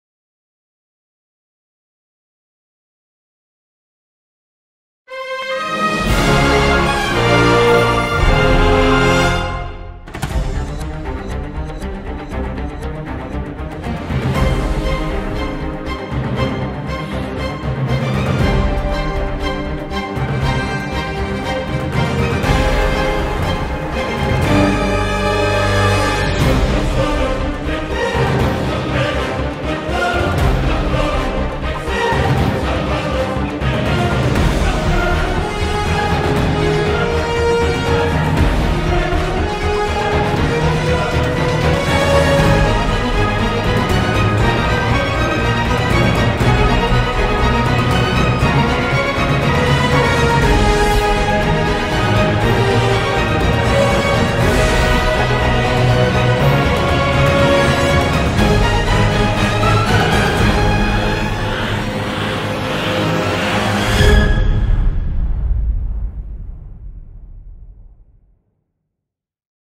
Anastasia the Maine Coon Kitten Trailer
A trailer for my new Maine Coon kitten coming to CFA show halls this fall 2014. I made this video when I was bored on my summer break and had a blast :) I love my new kitten. Thank you Donna Ensor!